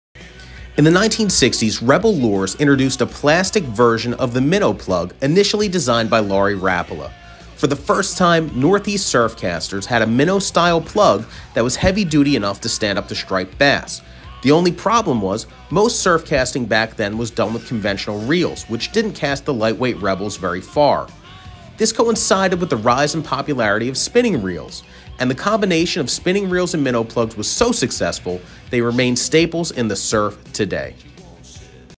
The Rebel Minnow: A revolutionary surf casting lure
The Rebel Minnow helped revolutionize surfcasting. Next time you catch a striper on an SP Minnow, be sure to thank a Rebel.